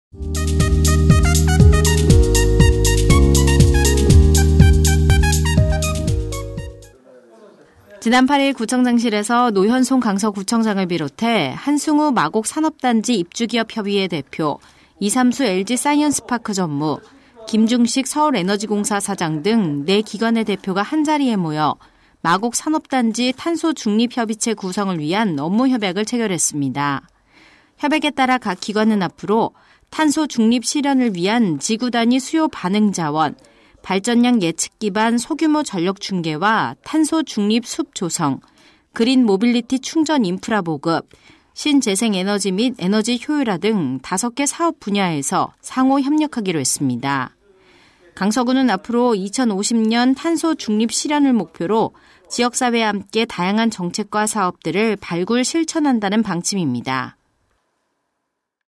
마곡산업단지 탄소중립 협의체 구성 업무협약식
지난 8일 구청장실에서 노현송 강서구청장을 비롯해 한승우 마곡산업단지 입주기업협의회 대표, 이삼수 LG사이언스파크 전무, 김중식 서울에너지공사 사장 등 네 기관의 대표가 한자리에 모여 ‘마곡산업단지 탄소중립 협의체 구성을 위한’ 업무협약을 체결했습니다.

협약에 따라 각 기관은 앞으로 탄소중립 실현을 위한 지구단위 수요반응자원(DR), 발전량예측기반 소규모전력중개와 탄소중립 숲 조성, 그린모빌리티 충전인프라 보급, 신재생에너지 및 에너지효율화 등 5개 사업 분야에서 상호 협력하기로 했습니다.

강서구는 앞으로 2050년 탄소중립 실현을 목표로 지역사회와 함께 다양한 정책과 사업들을 발굴, 실천한다는 방침입니다.